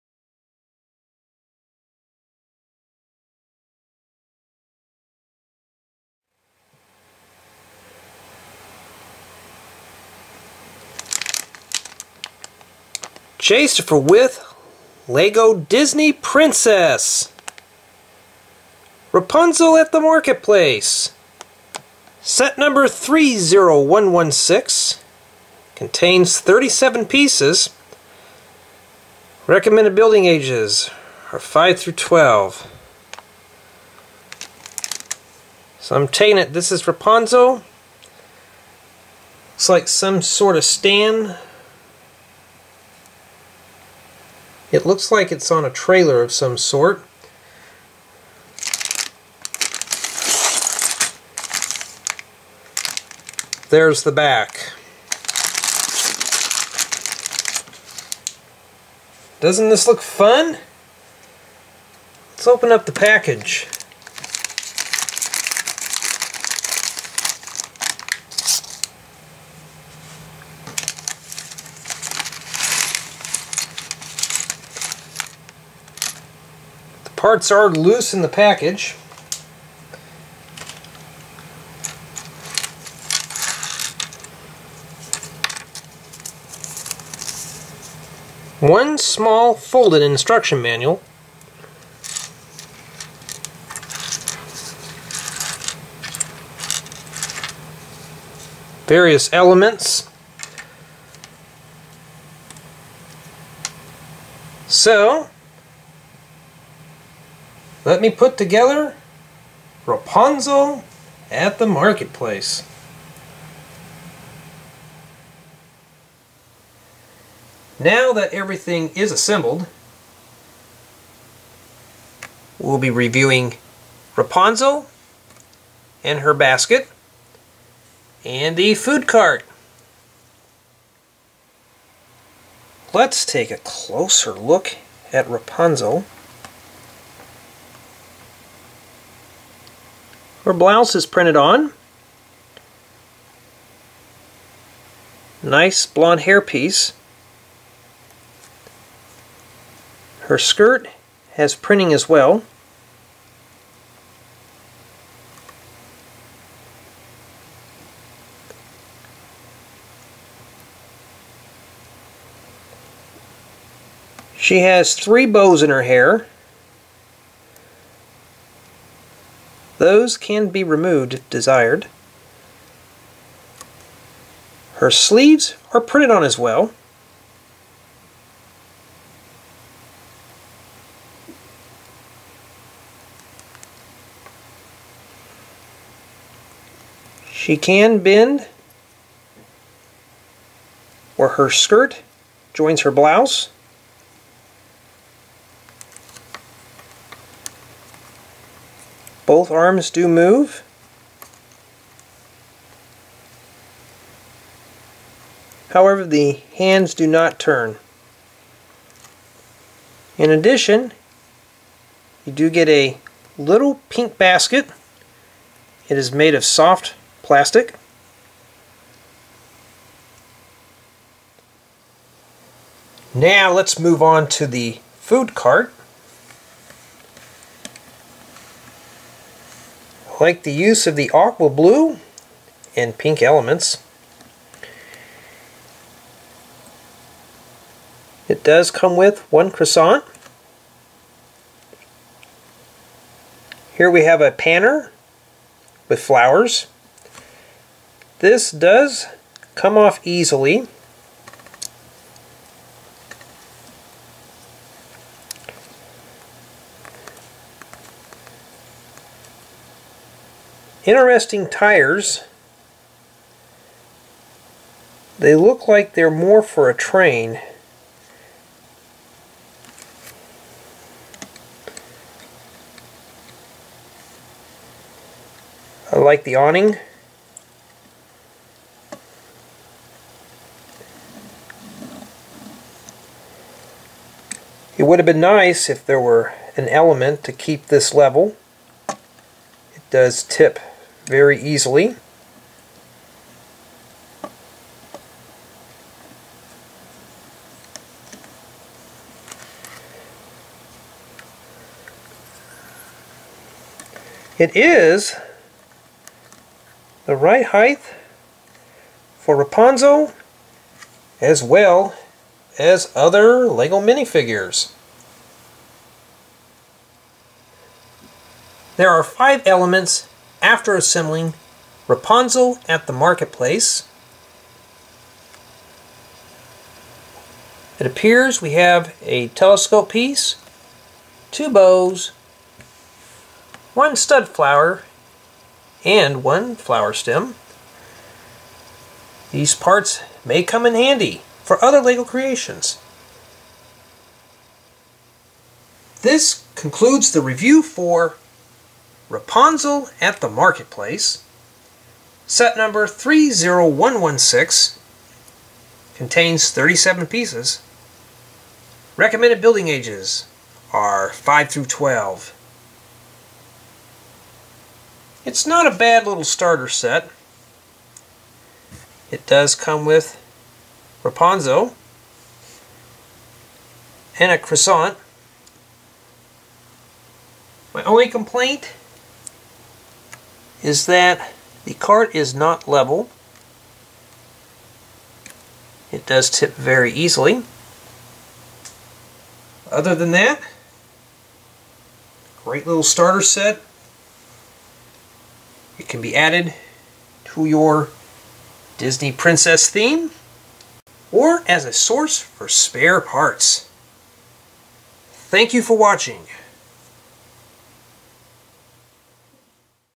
LEGO Disney: Rapunzel At The Marketplace 30116 Exploration
An exploration of Rapunzel At The Marketplace by LEGO Disney 30116. The set contains 37 pieces which includes Rapunzel and parts to make the food cart. The retailed for 3.99 dollars at the time of this recording.

JAY is an AFOL LEGO hobbyist, LEGO builder, and LEGO collector who enjoys building with LEGO bricks in his spare time.